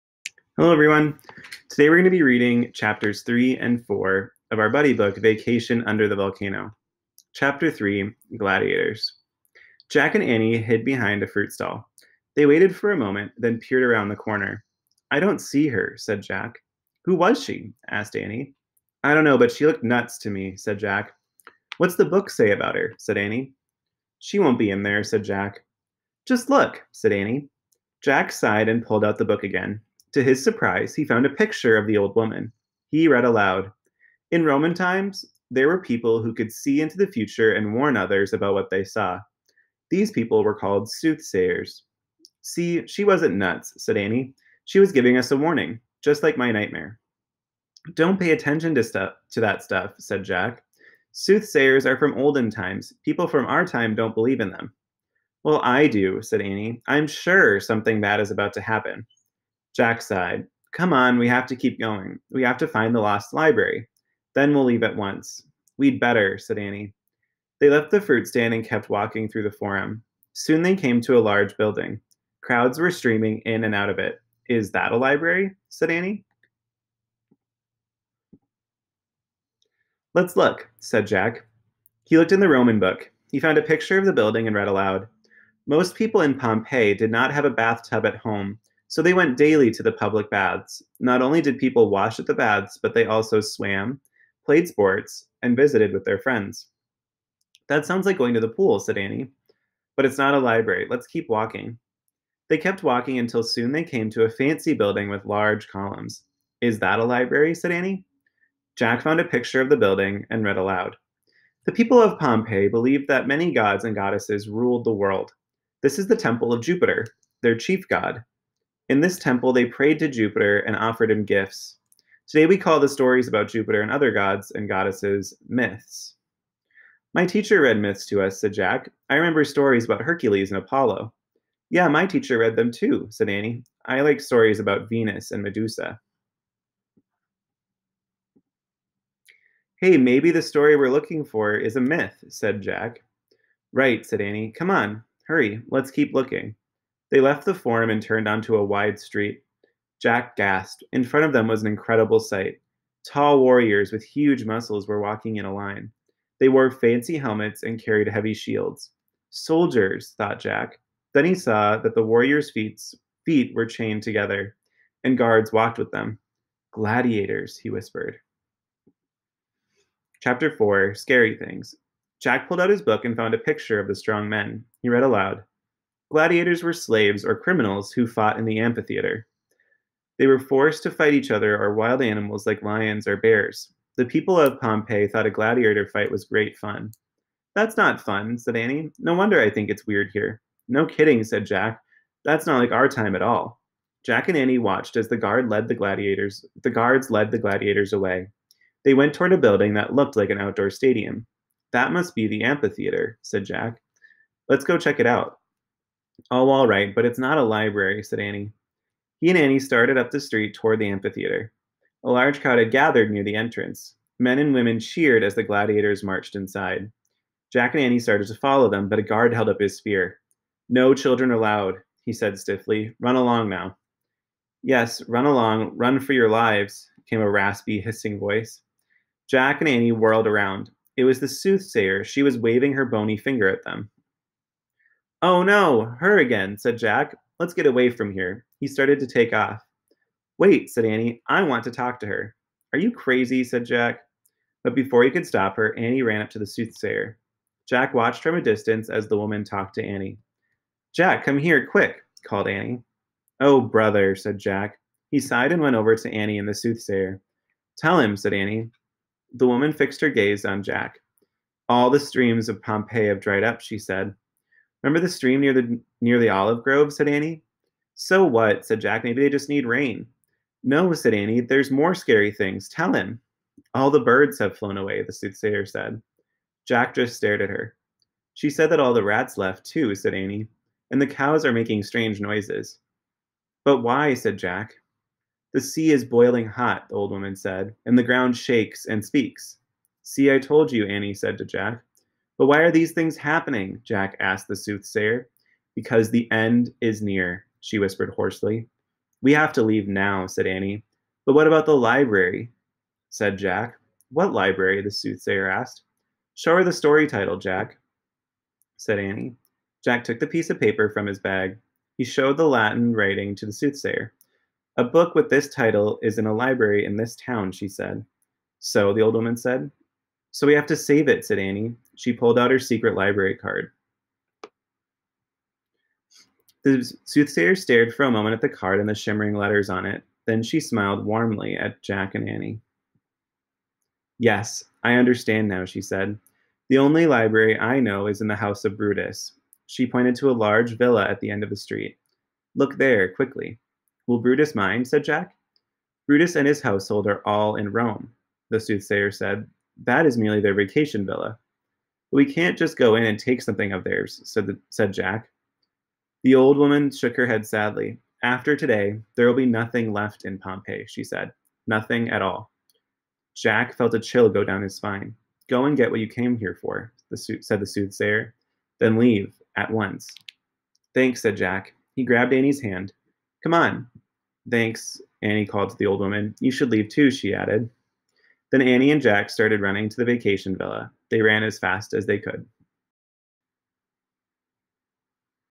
(4.28) Chapters 3 and 4 of Vacation Under the Volcano
Here are chapters 3 and 4 of our buddy book Vacation under the Volcano.